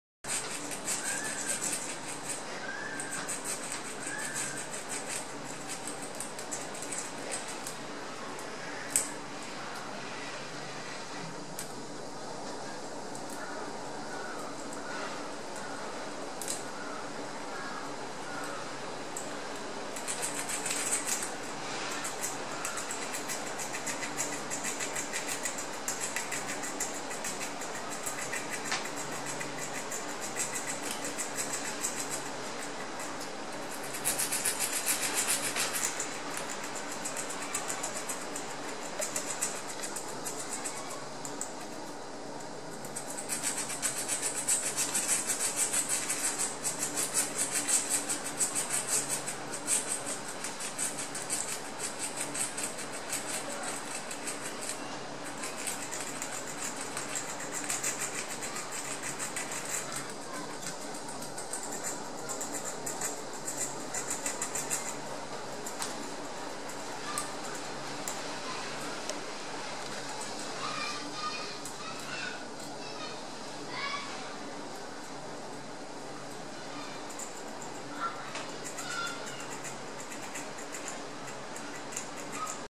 どさんこの森 くるみ割り2重奏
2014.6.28　札幌市円山動物園。　「どさんこの森」のエゾリスたちが久しぶりに胡桃のおやつを貰っておりました。でも相変わらず割るのは下手くそみたいです。。